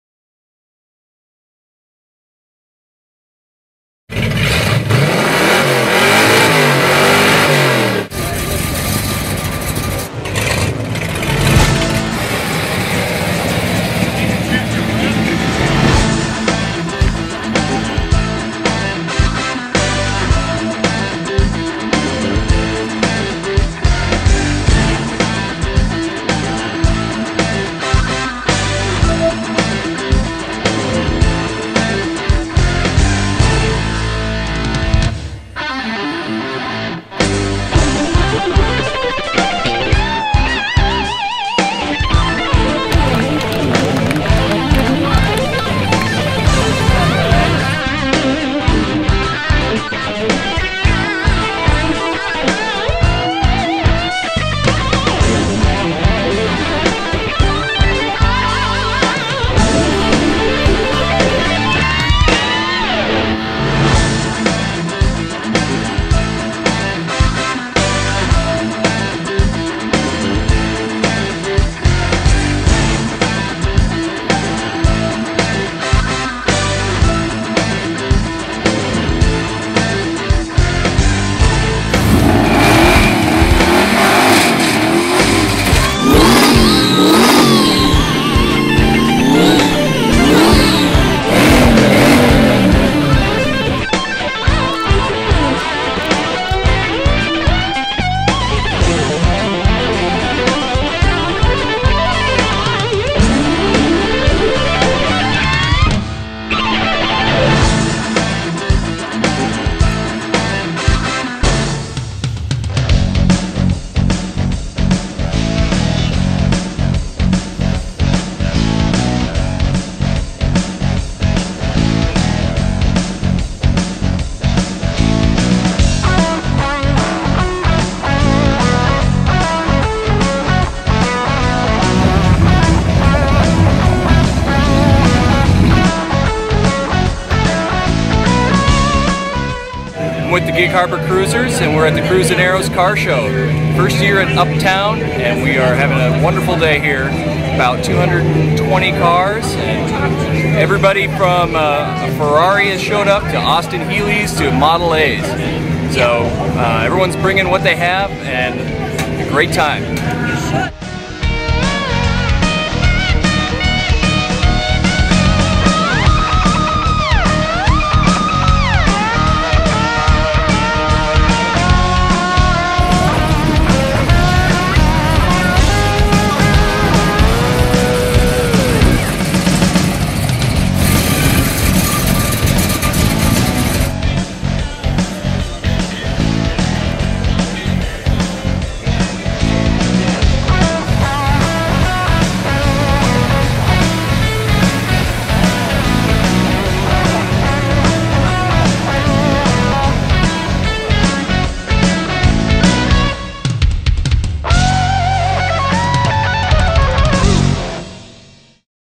Gig Harbor Cruisers: Uptown Car Show
"Uptown Car Show" Annual event in Gig Harbor. Part of a news story from Gig Harbor Life (GigHarbor-Life.com)